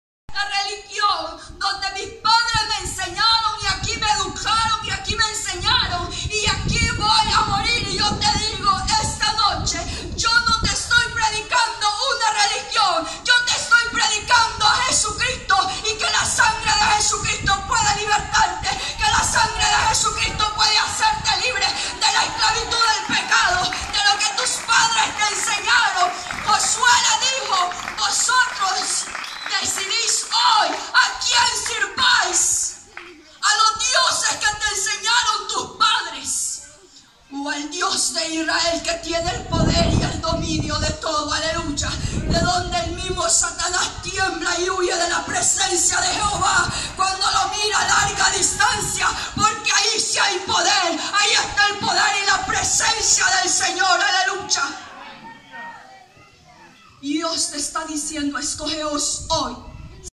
Prédica en San Fernando, Chalatenango
Por primera vez en San Fernando, llevando la palabra cantada y explicada. Dios bendiga a sus habitantes, esa palabra no regresará vacía